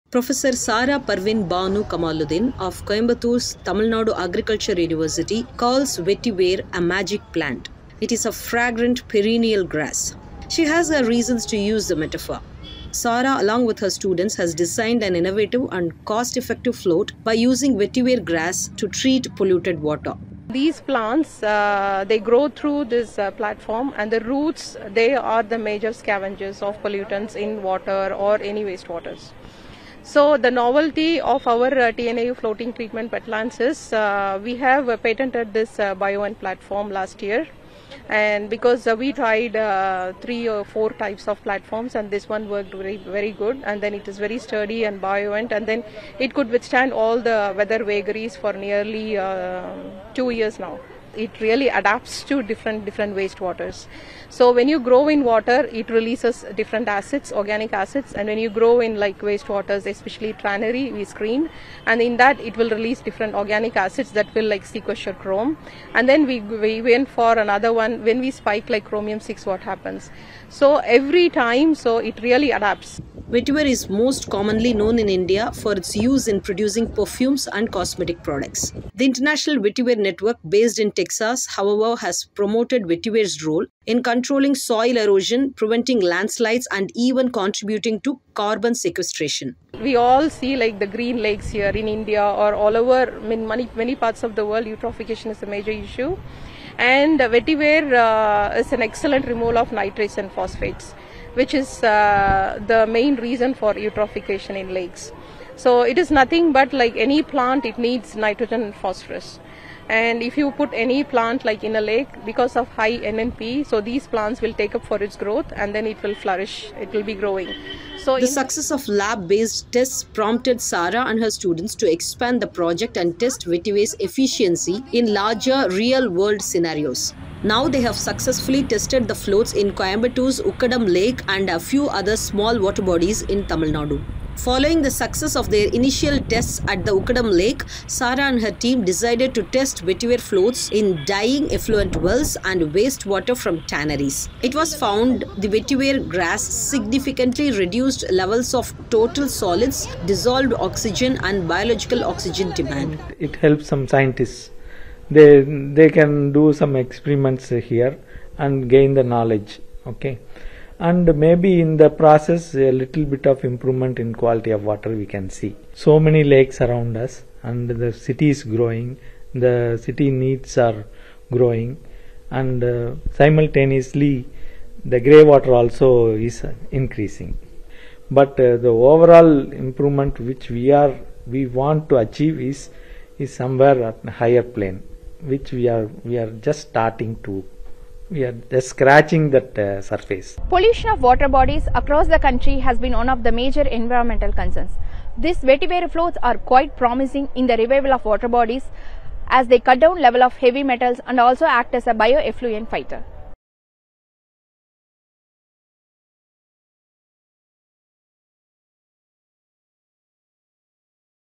Vetiver grass, an eco-friendly alternative to treating sewage
Discover how Professor Sara Parwin Banu Kamaludeen and her team at Tamil Nadu Agricultural University (TNAU) are using Vetiver grass to purify water. From treating sewage water to tackling environmental issues like eutrophication, Vetiver has proved to be a ‘magical’ plant. In the video, we dive into the cutting-edge research led by Professor Sara and her students, who designed a floating Vetiver platform to clean contaminated water. Learn how this eco-friendly and cost-effective solution is making waves in water pollution control, including its successful application in Ukkadam Lake in Coimbatore.